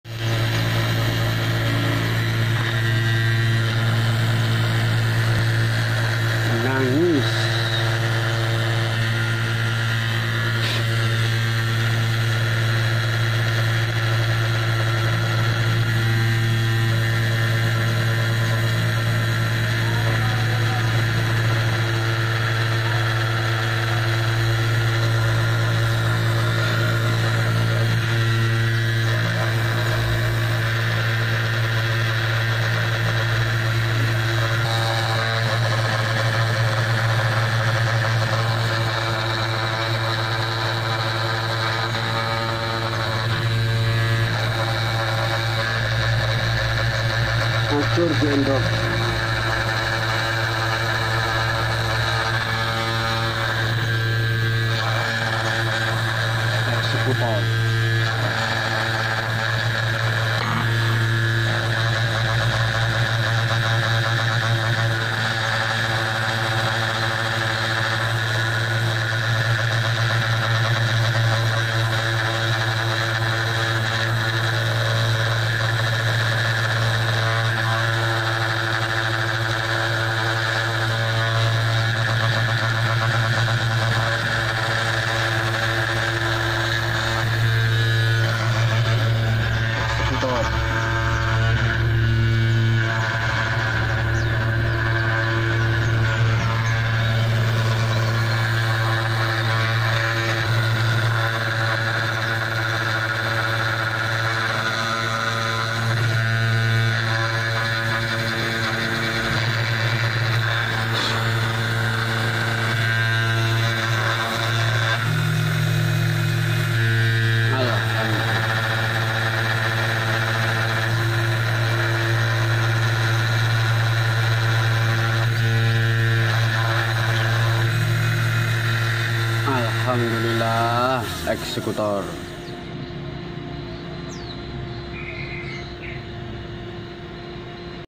Grafir Chamber CNC 2417
Grafir Chamber Senapan, anodize ungu

spindle  motor 775 Rpm 6000
feedrate : 1.4 mm/sec
plunge rate : 0.4 mm/sec
end mill carbide
ballnose R0.3 mm

makasih telah menonton jangan lupa